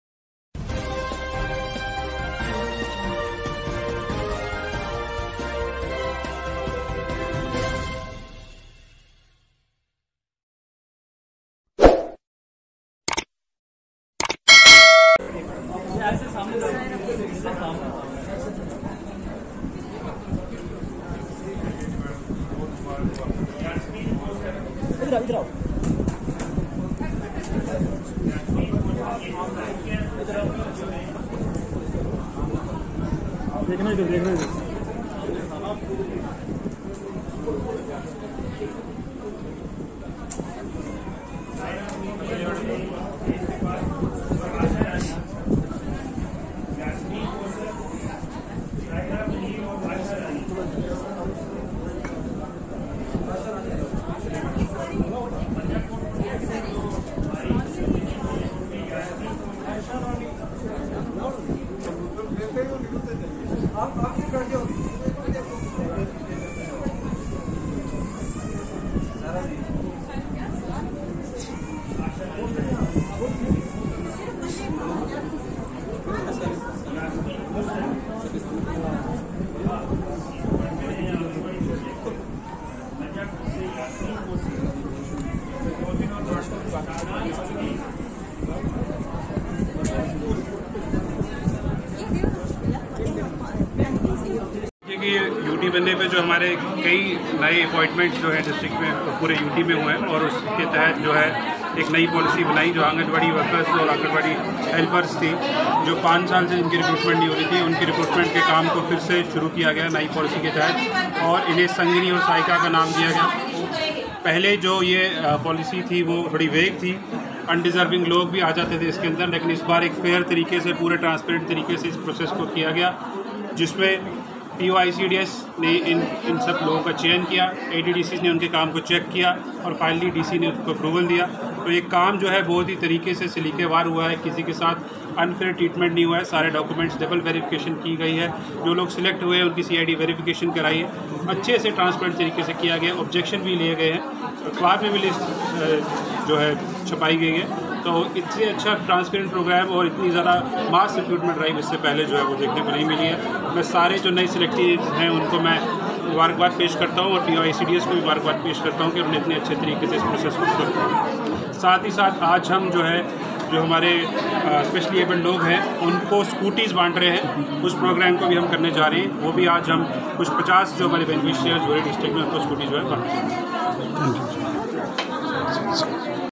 DC Rajouri Vikas Kundal distributed the appointment letter to newly Anganwadi Workers/Helpers
DC Rajouri Sh Vikas Kundal, IAS along with ICDS officer Rajouri/poonch Showkat Mehmood Malik,(KAS), Addl. DC and ACR Rajouri  distribution the appointment letter to newly selected Anganwadi Workers/Helpers.